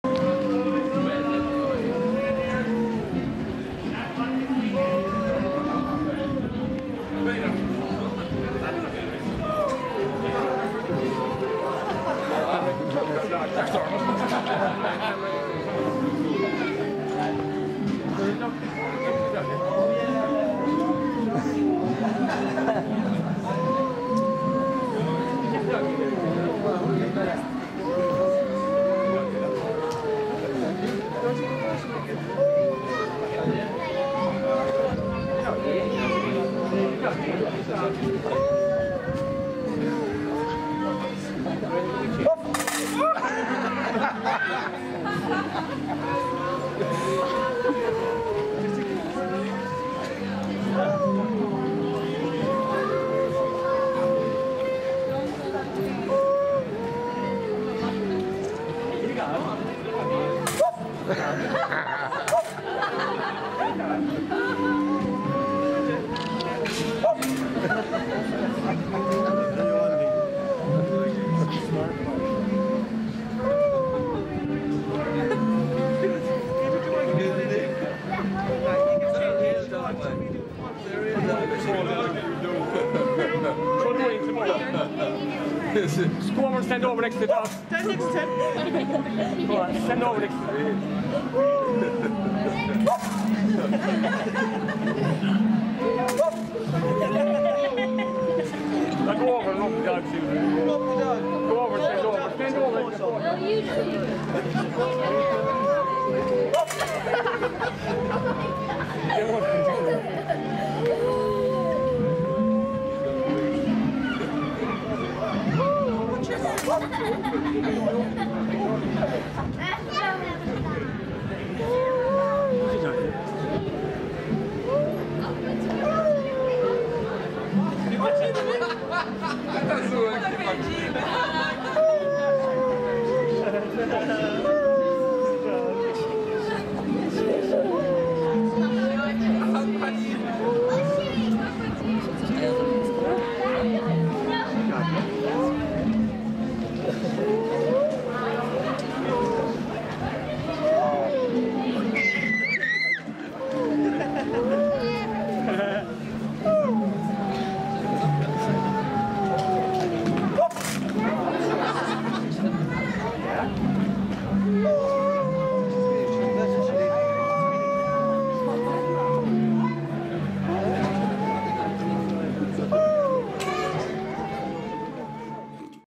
Incredible street performer - Funny singing dog 😁🤣😂
Incredible street artist, we were crying with laughter